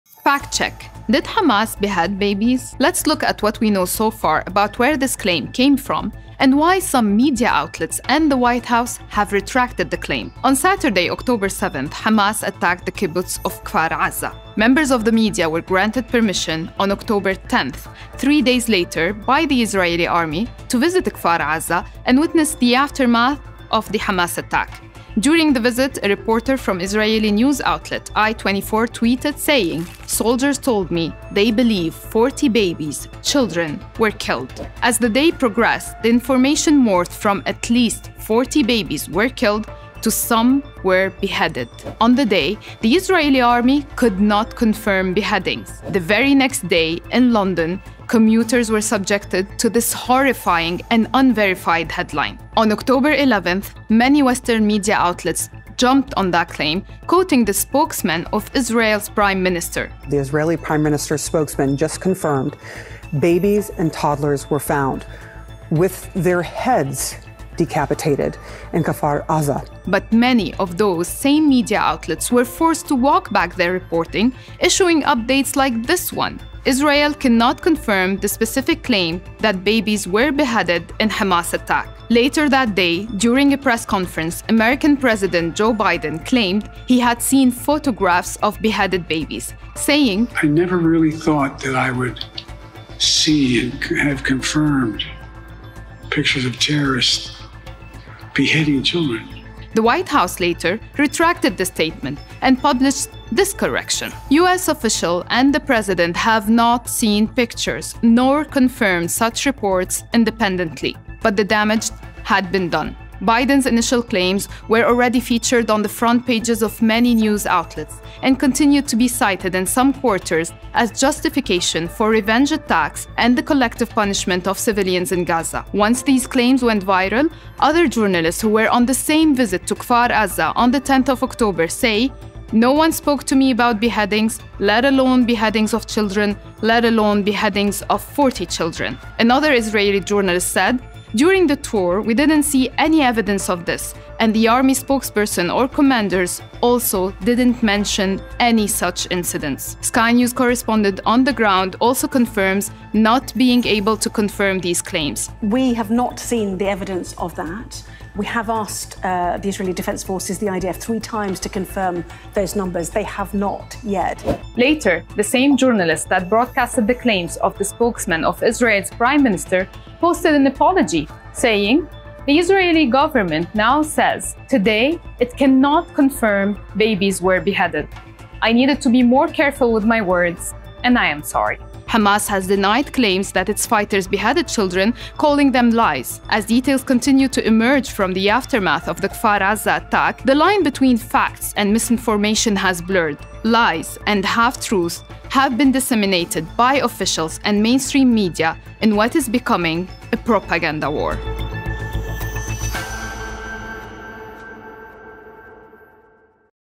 Did Hamas behead babies? I Fact Check
In this Fact Check, Al Jazeera's @rawaak tracks the origin and the spread of fake claims about the beheading of children in Kfar Aza.